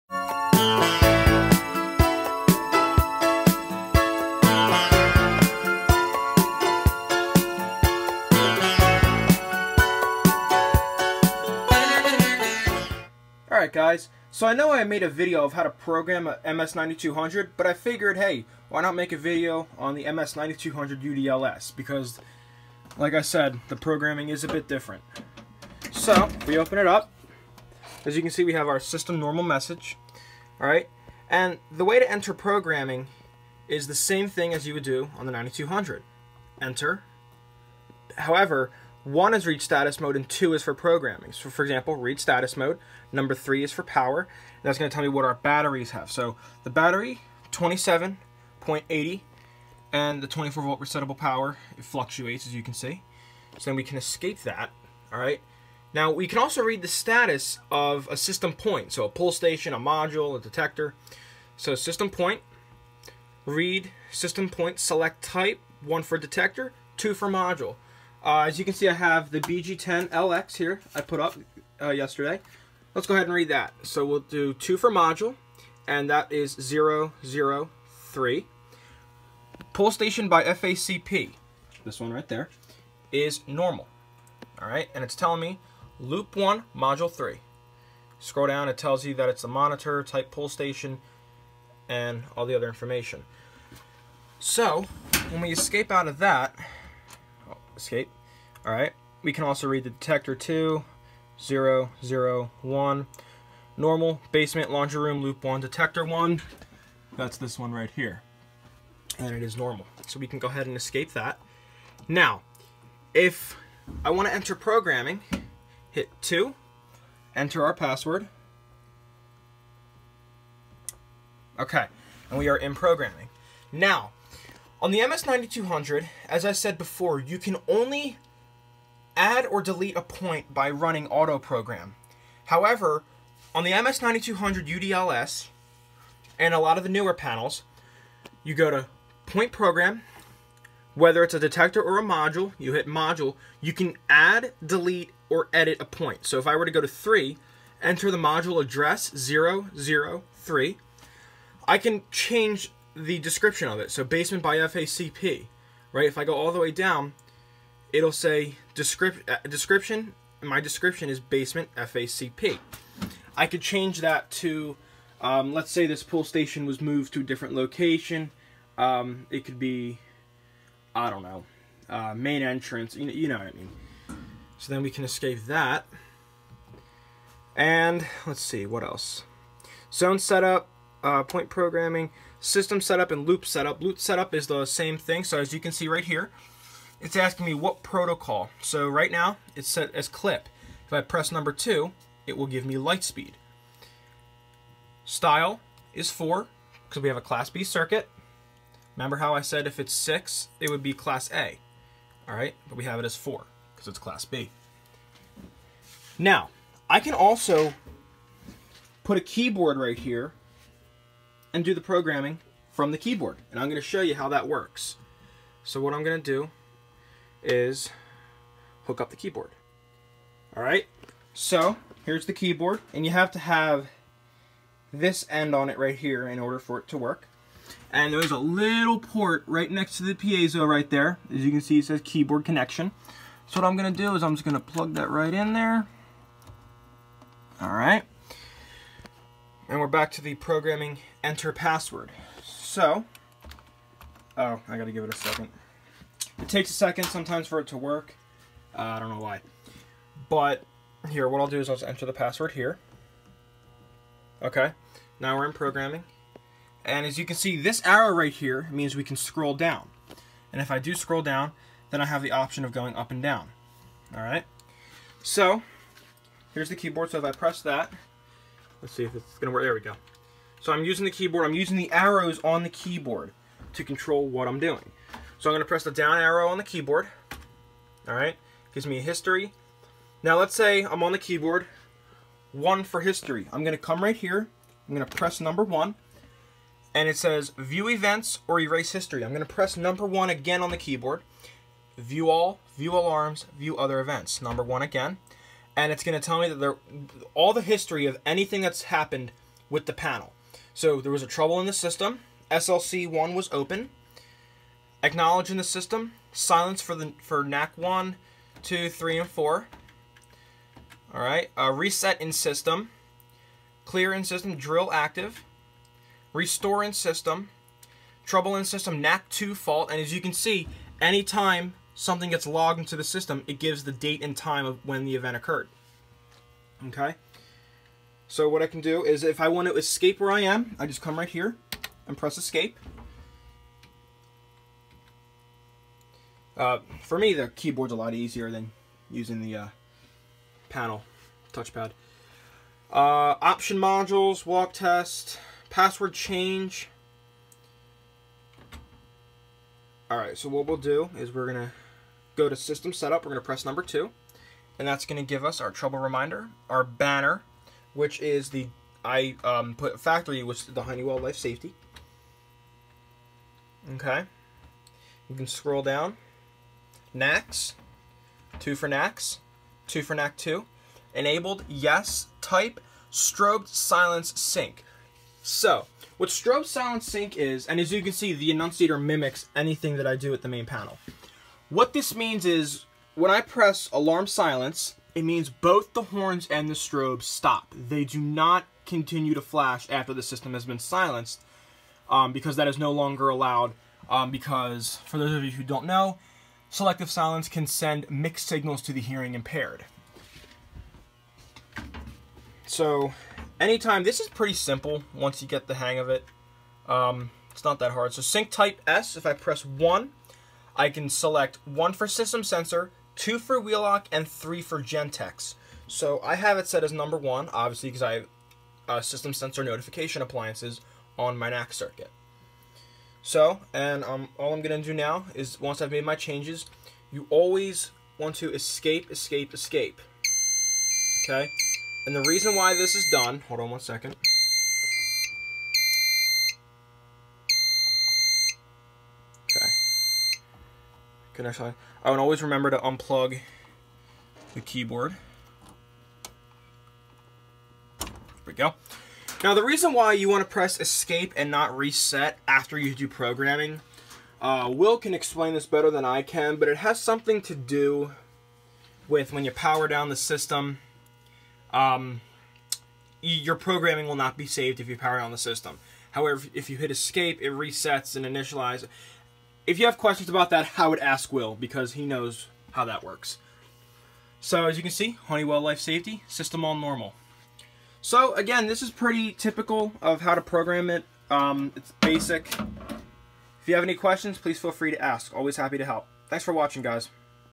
How to program a Fire Lite MS-9200UDLS
In this video, I will be teaching you basic programming on the Fire Lite MS-9200UDLS. Hope this helps! Enjoy!